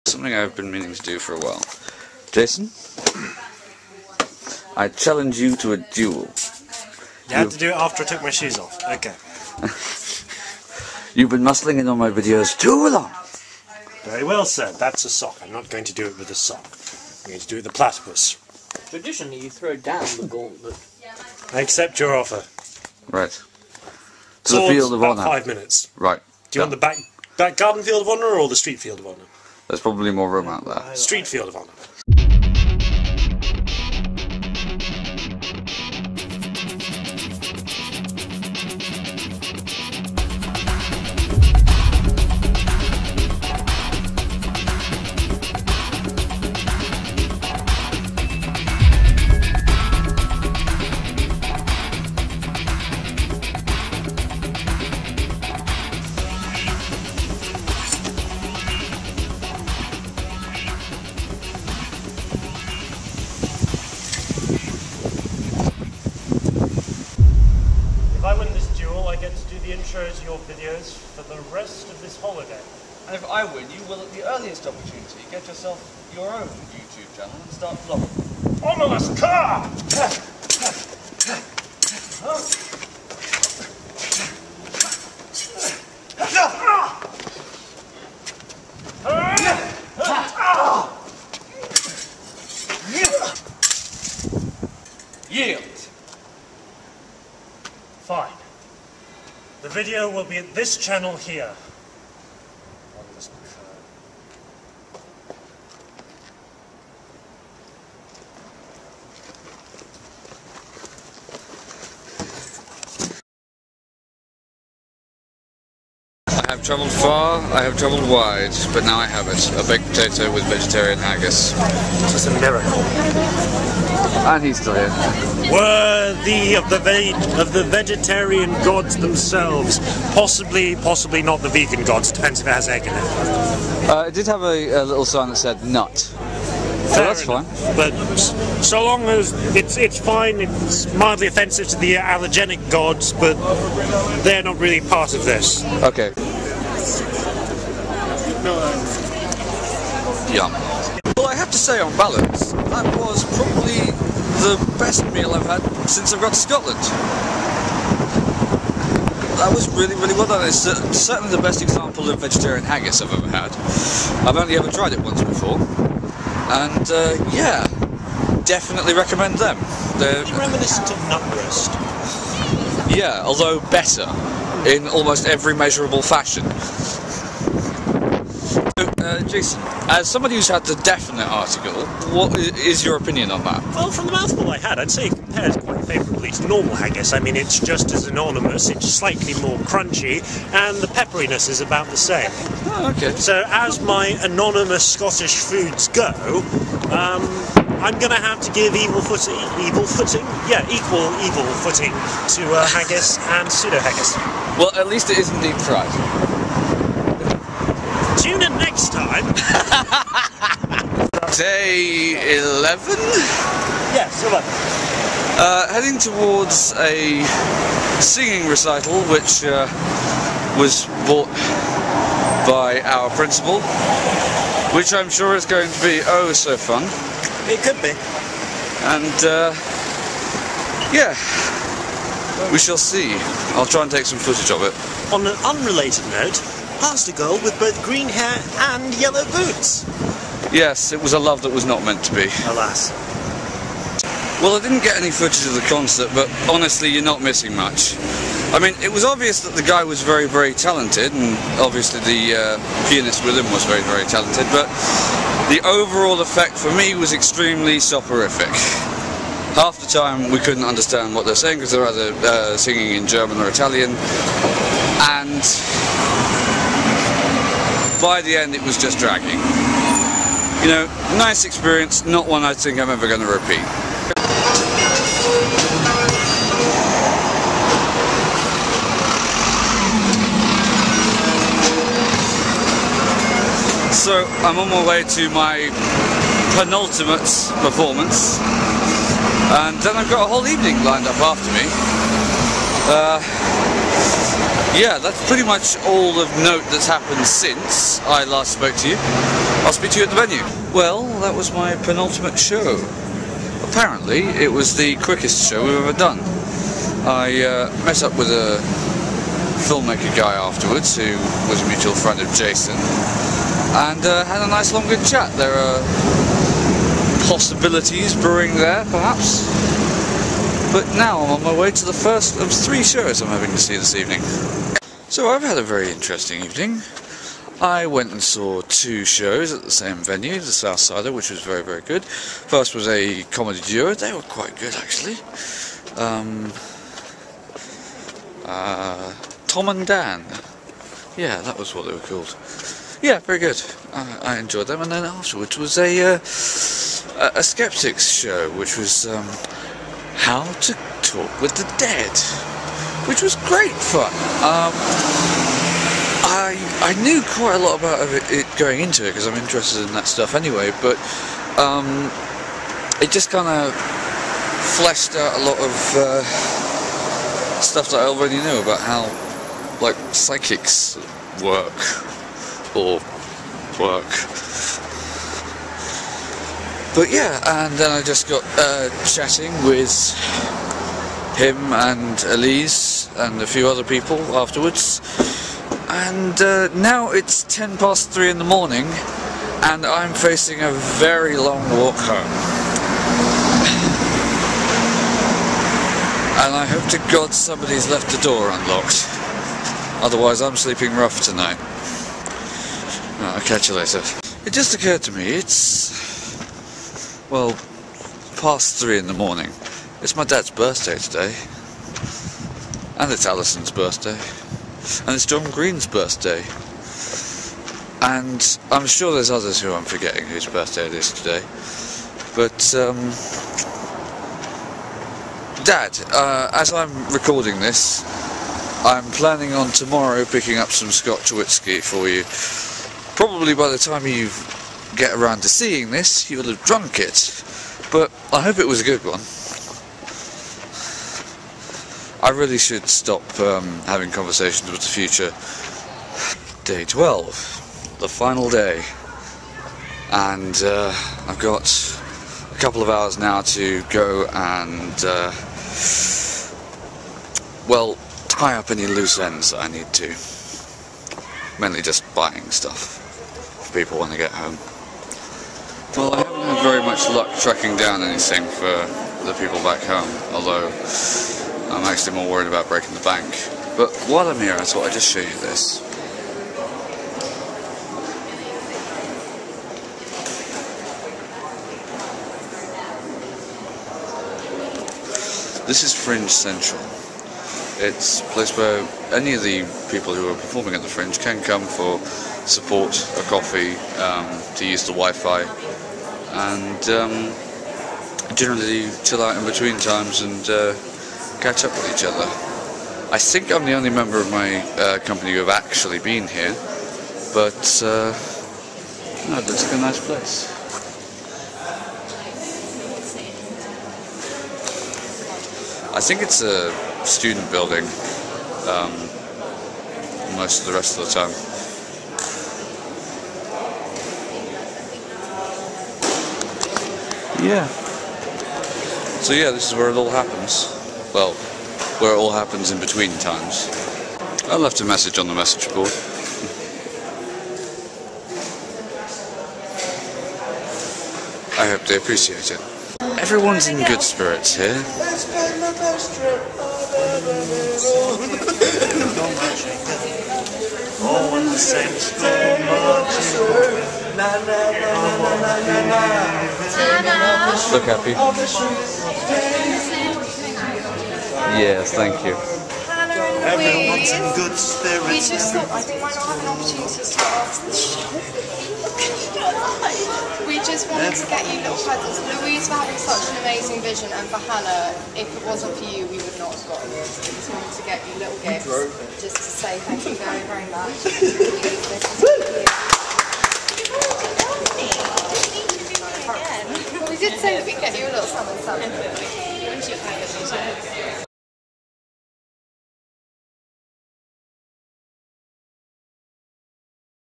Edinburgh Fringe Diary Day 11 & 12
Beginning with dual of epic proportions over who gets to do the intros to my videos. Thense on to Vegetarian haggis. Food of the Gods. A few more reviews and loose ends tied up.